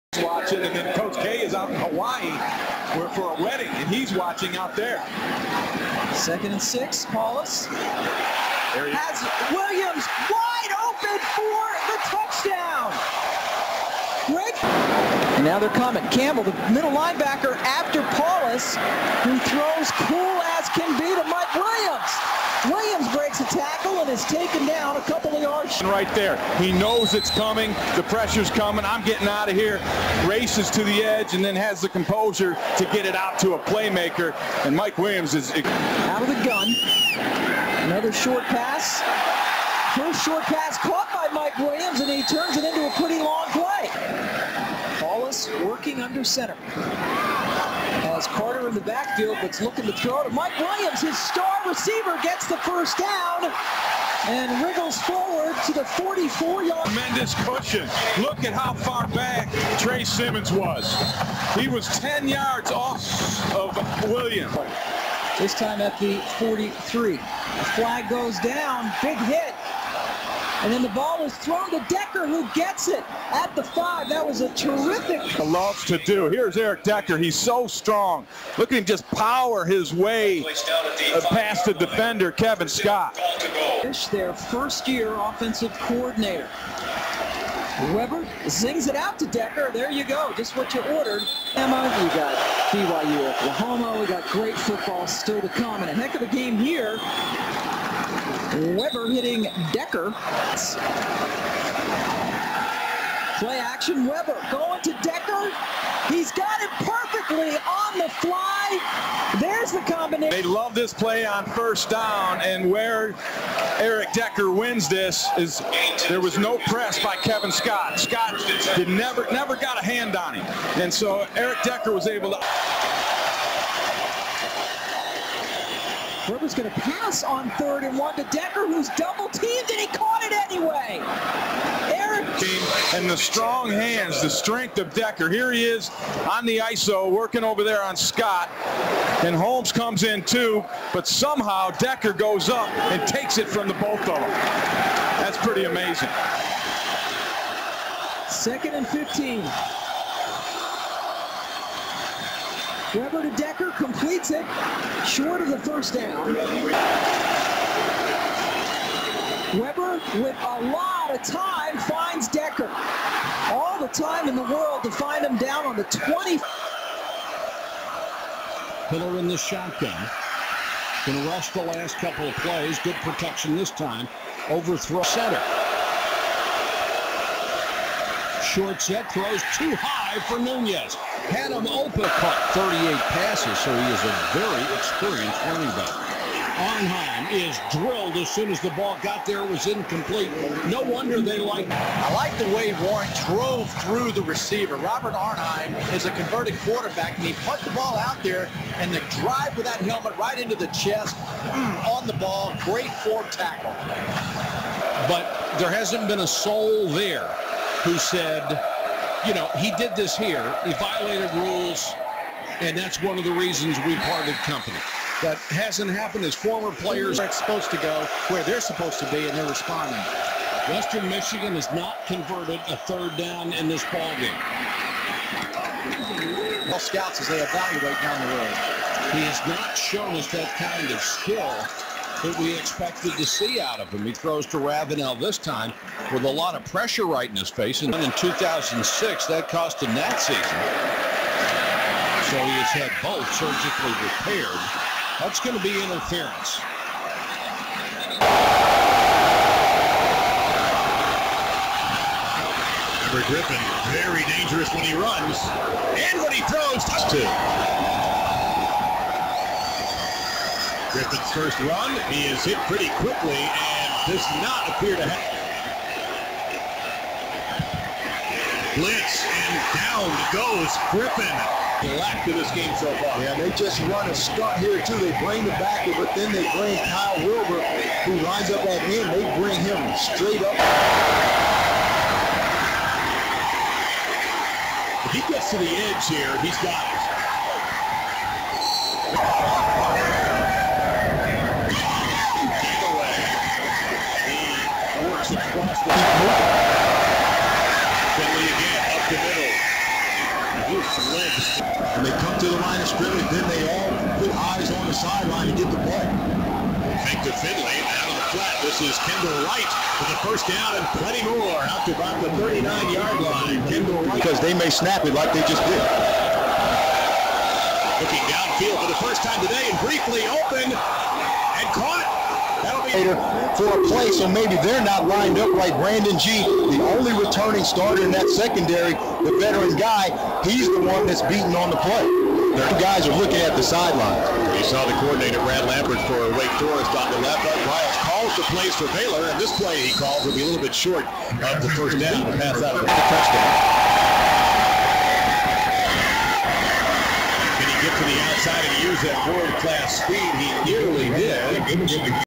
DraftParty's Week 1 Highlights/Lowlights Part 6 2009
ProDraftParty.com
1. WR Mike Williams vs Minnesota
2. WR Eric Decker vs Syracuse
3. CB Donovan Warren vs WMU
4. DT Boo Robinson vs Baylor
5. CB Brandon Ghee vs Baylor